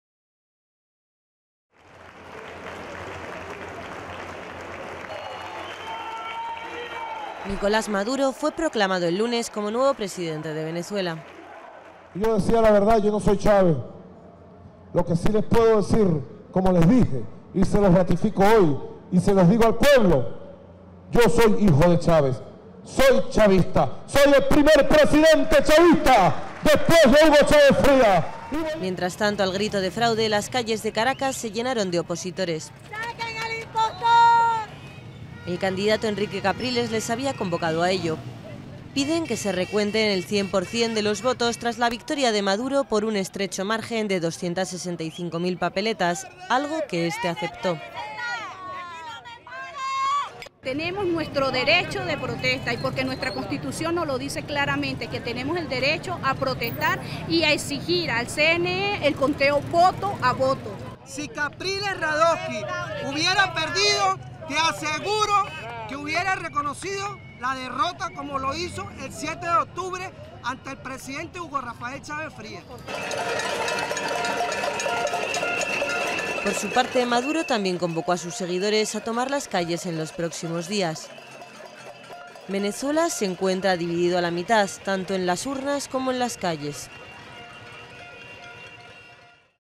Protestas en Caracas
Mientras Nicolás Maduro es proclamado presidente de Venezuela, las calles de Caracas se llenan de convocados por el candidato Henrique Capriles que piden el recuento de los votos tras el estrecho margen por el que ganó el chavismo.